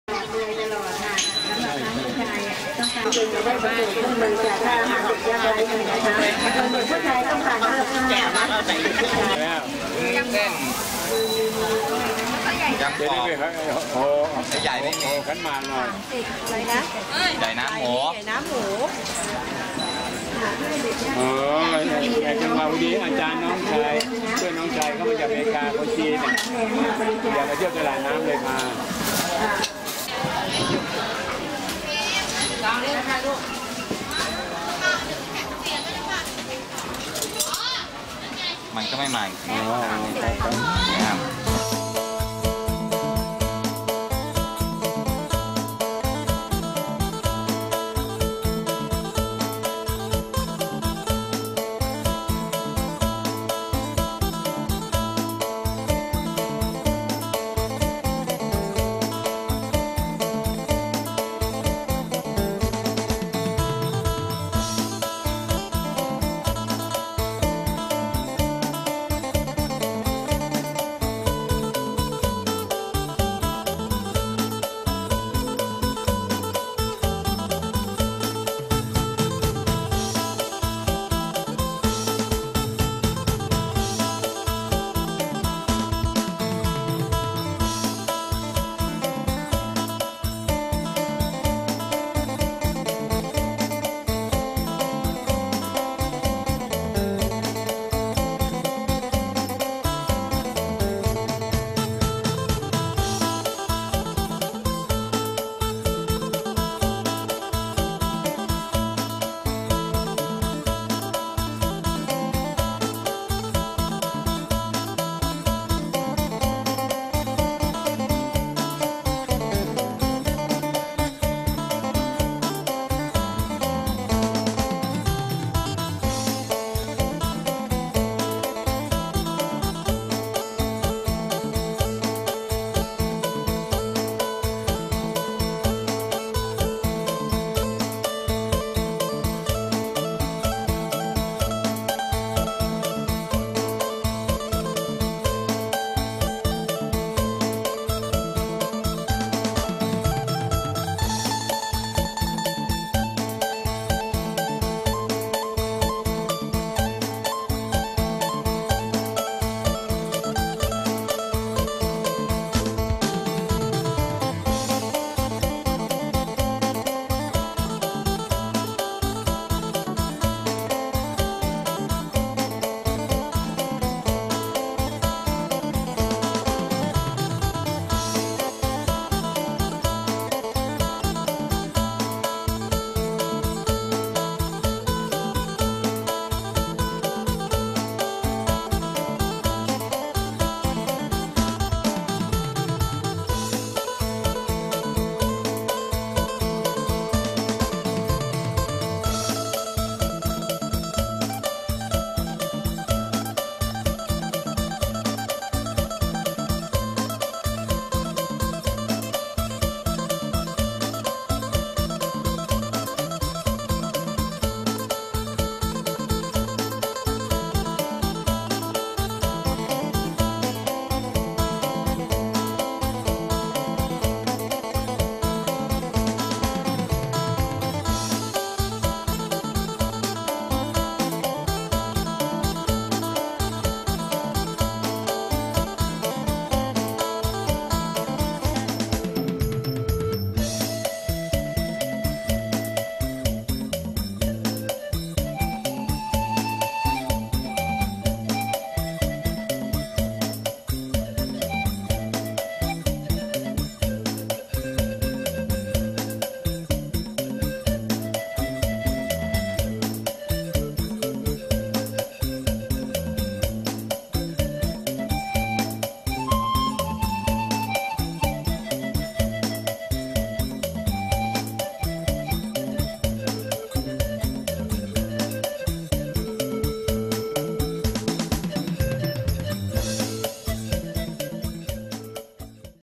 Street Food in Bangkok - 2011
Street food in Bangkok opens your eyes and mouths.
Chath Kovitchindachai, Manoch Kongkathip, Manoch's brother and Somkiat opened doors for me.